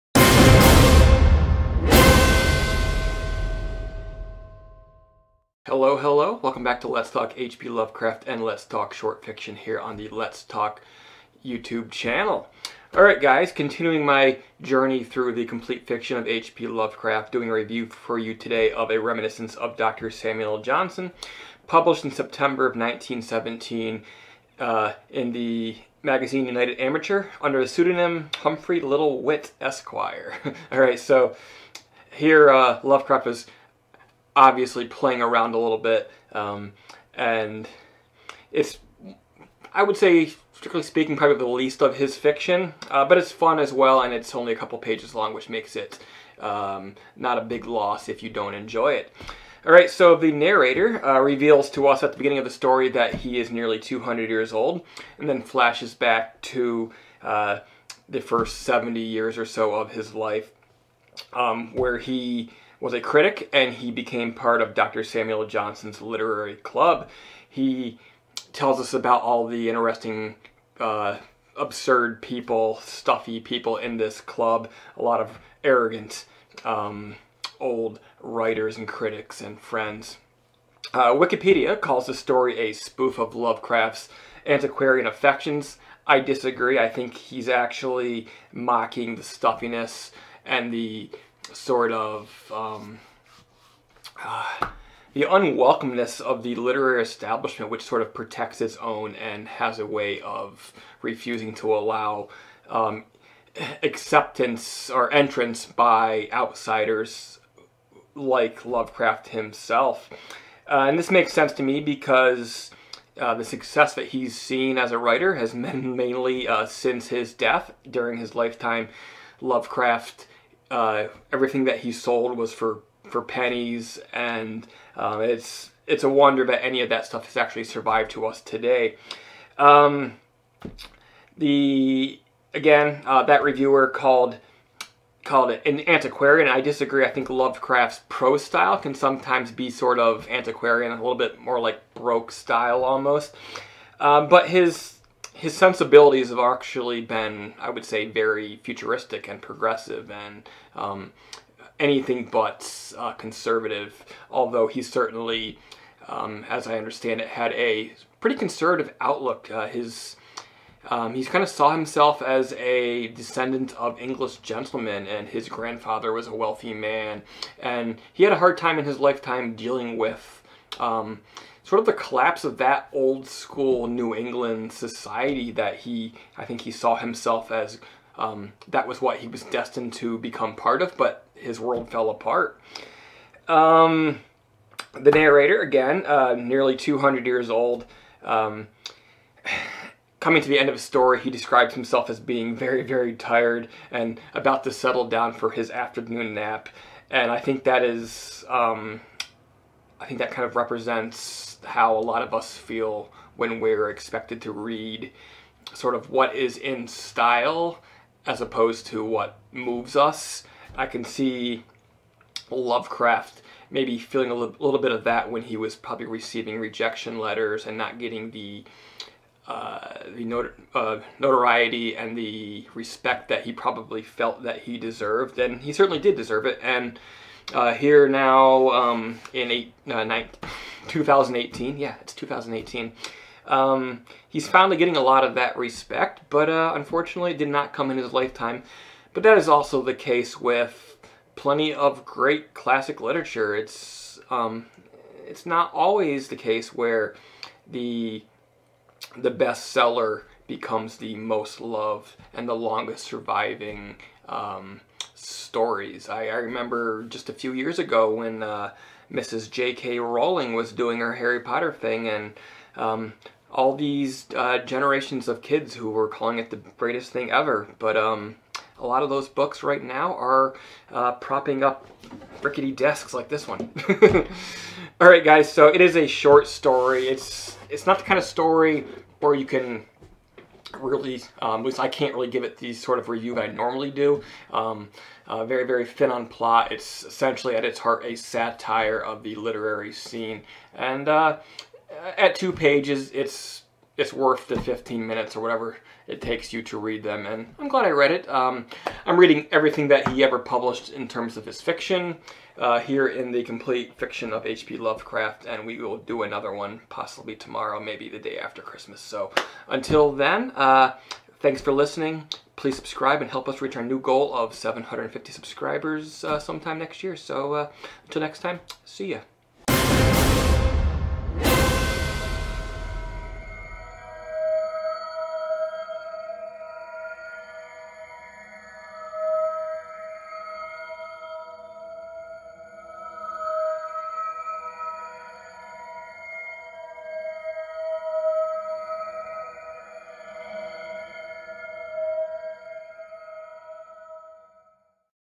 A Reminiscence of Dr. Samuel Johnson (Review)
"A Reminiscence of Dr. Samuel Johnson" is a short story written in 1917 by American horror fiction writer H. P. Lovecraft. It was first published in the September 1917 issue of the United Amateur, under the pseudonym Humphrey Littlewit, Esq.

"Howard Phillips Lovecraft was an American writer of weird, science, fantasy, and horror fiction. He is best known for his creation of the Cthulhu Mythos. Born in Providence, Rhode Island, Lovecraft spent most of his life in New England."